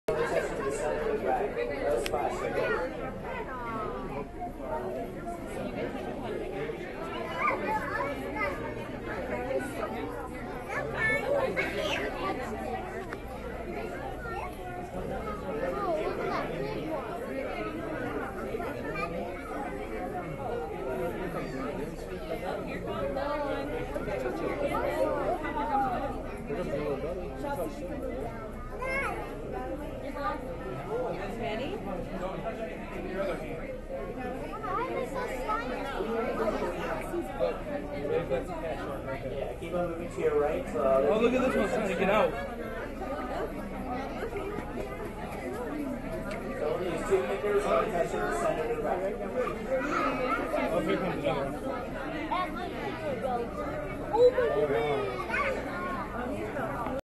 Hands-on Tank in 360° from the Toledo Zoo Aquarium
This was some 360° video captured from the edge of the hand-ons tank at the Toledo Zoo's Aquarium.